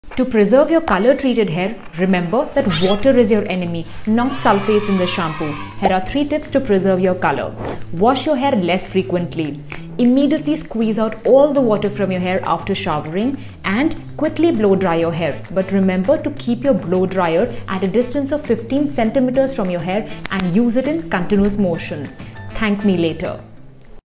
Has your salon pitched an expensive shampoo to preserve your colour treated hair? Dr Jushya Bhatia
Have you been told that sulphate-free shampoo would preserve your hair colour? 🫢 Let me tell you, water is your enemy and not the sulphates in your shampoo 🤐

Here are 3 ways you can protect your hair from water damage 💧

1. Wash your hair less frequently 🚿 Water makes your hair swell and not sulphates. This swelling can create pores that will easily remove your hair colour 😐

2. Post your shower immediately squeeze out all the water from your hair 🫧

3. While choosing your shampoo 🧴, look for Amomethicone, it really helps preserve your hair colour ⛑

4. Don’t wait for too long before blow drying your hair. However, I advise using heat protectants while using heating devices 👍🏻

Pro tip- Use your dryer at a distance of 15 cm from your hair and use in continuous motion.

Here’s to happy hair days 🍺

For Appointments -

📫 Sarin Skin Solutions
  D-396, Defence Colony, New Delhi